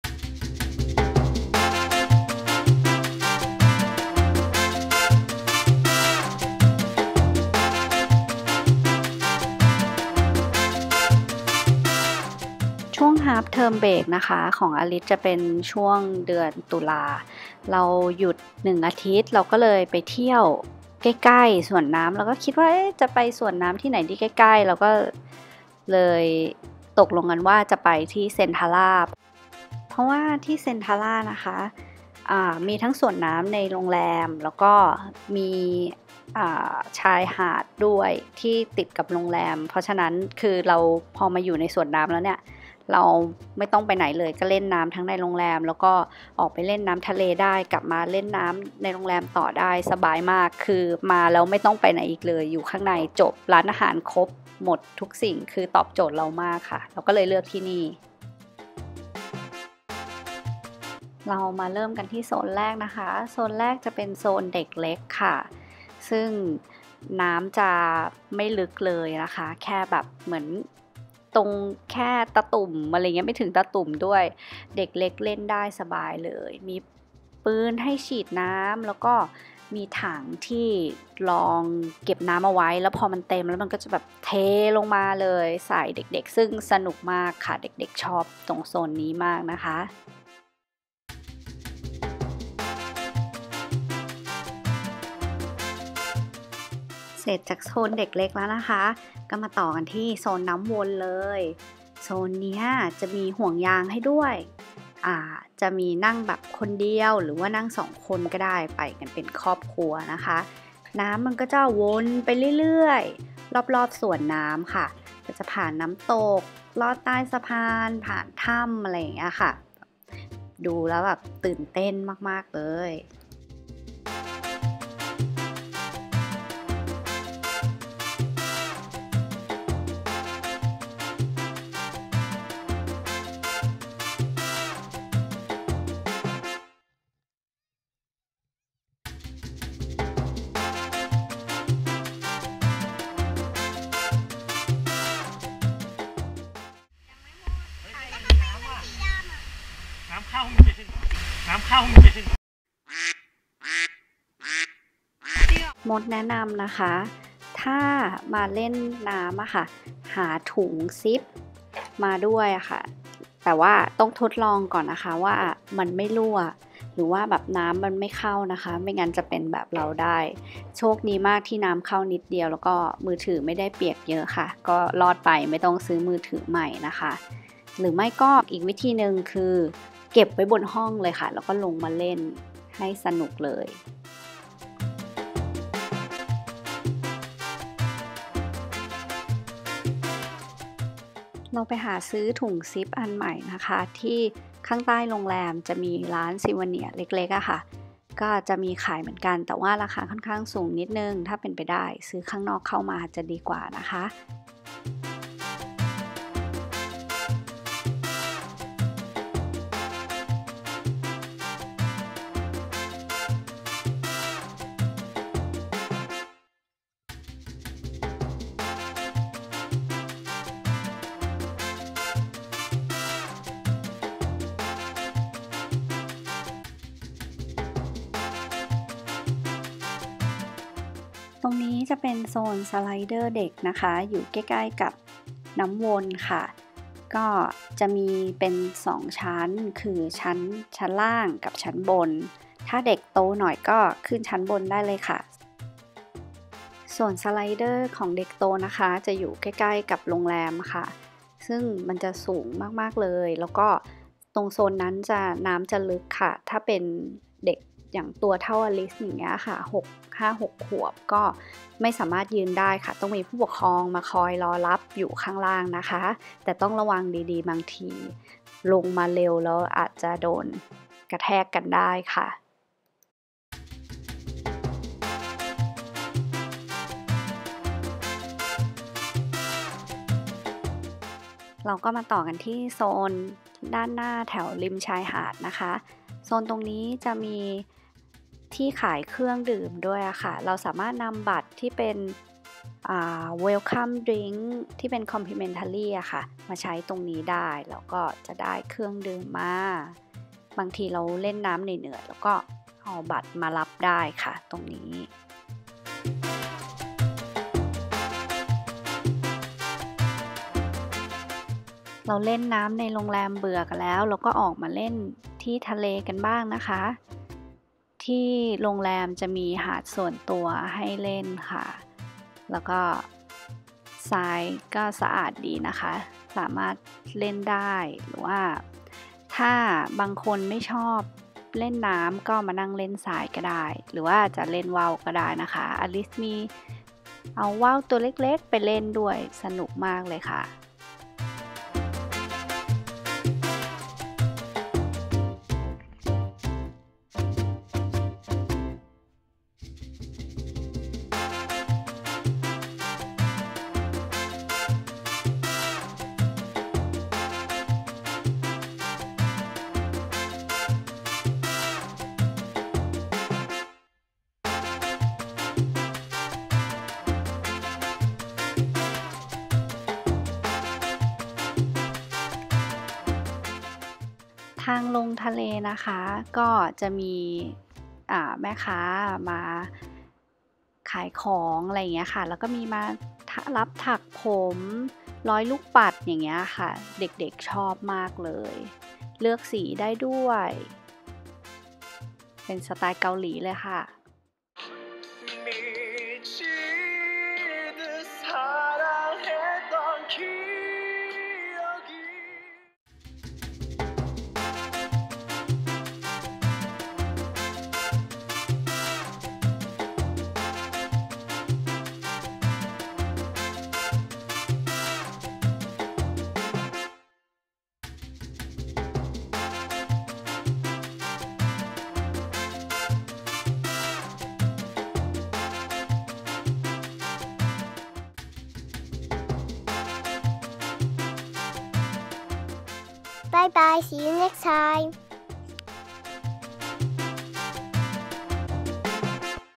เที่ยวสวนน้ำเซ็นทารา | Centara Grand Mirage Beach Resort Pattaya
ช่วง Half term break เดือนตุลาคม จะหยุดแค่ 1 อาทิตย์ เราเลยคิดกันว่าจะไปเที่ยวไหนดีที่ใกล้ๆกทม. โรงแรมCentara ตอบโจทก์เรามากค่ะ เพราะมีสวนน้ำและมีหาดส่วนตัว แถมใกล้กทม.อีกด้วย
ทริปนี้อลิซสนุกมากค่ะ